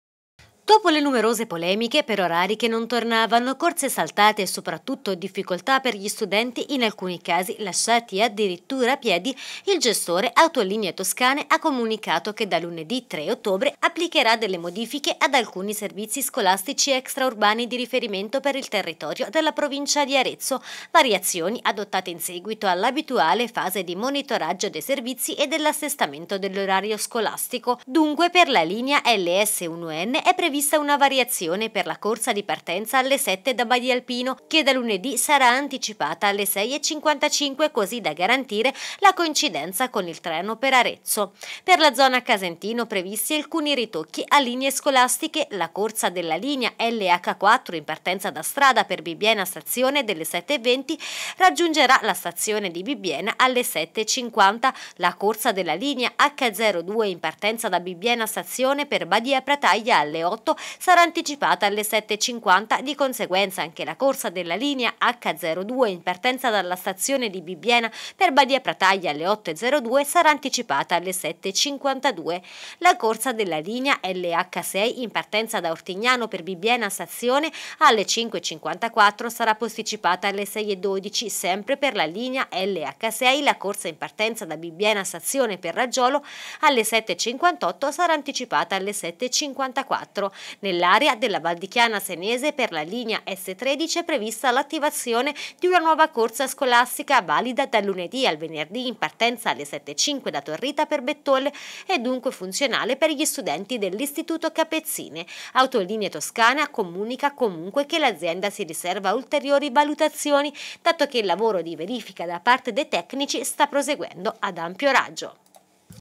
Autobus, dopo le polemiche arrivano le variazioni decise da Autolinee Toscane
Da lunedì 3 ottobre Autolinee Toscane applicherà delle modifiche ad alcuni servizi scolastici extraurbani di riferimento per il territorio della provincia di Arezzo. Le variazioni sono state adottate in seguito all’abituale fase di monitoraggio dei servizi e all’assestamento dell’orario scolastico. Quelle di seguito sono le linee interessate da questa prima fase ma l’azienda si riserva ulteriori valutazioni, dato che il lavoro di verifica da parte dei tecnici sta proseguendo ad ampio raggio.